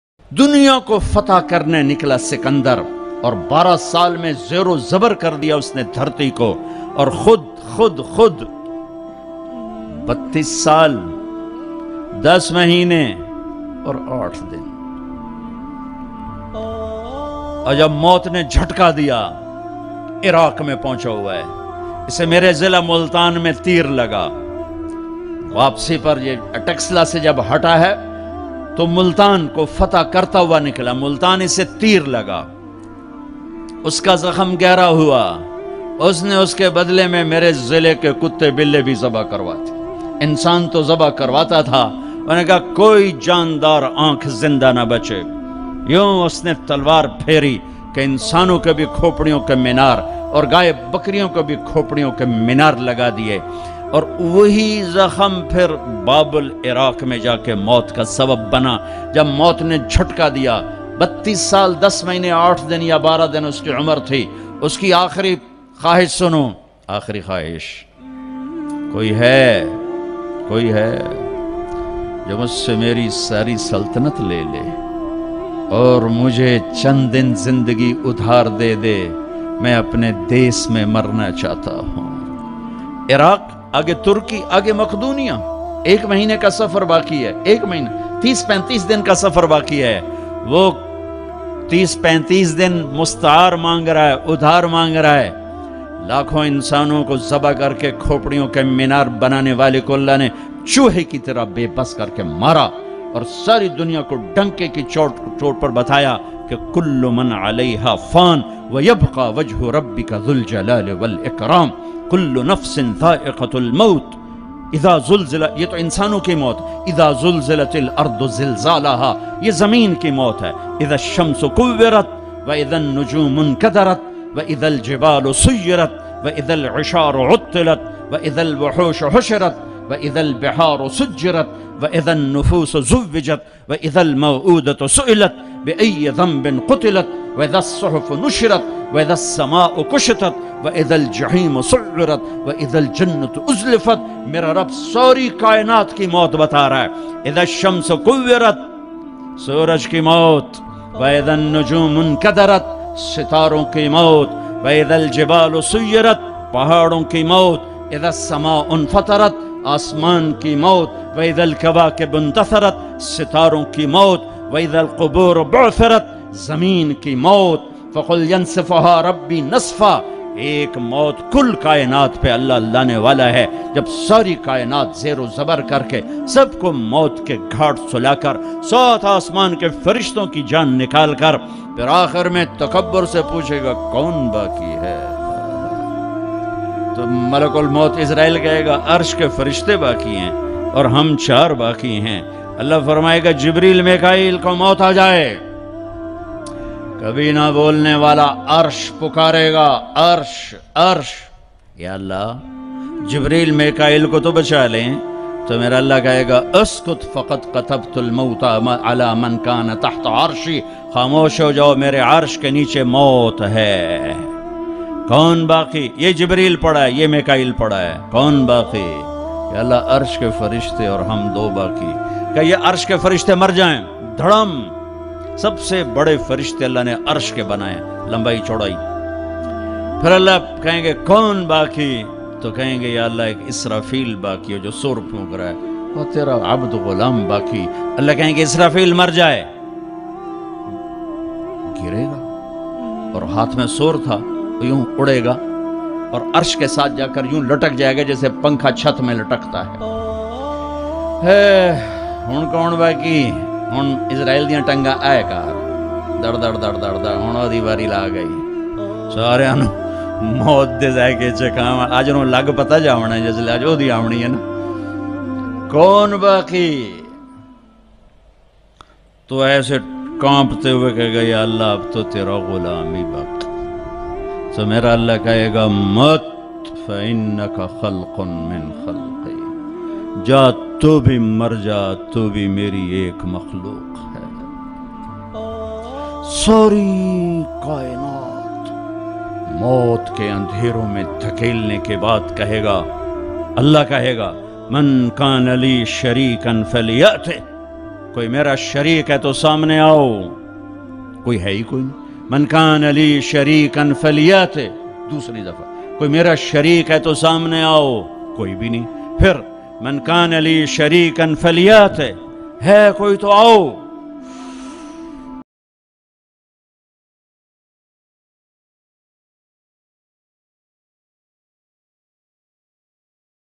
Sikandar e Azam Death History in Urdu سکندر اعظم By Molana Tariq Jameel M H
sikandar history old dua tariqjamilstatus duawhatsapp status maulanatariqjameel .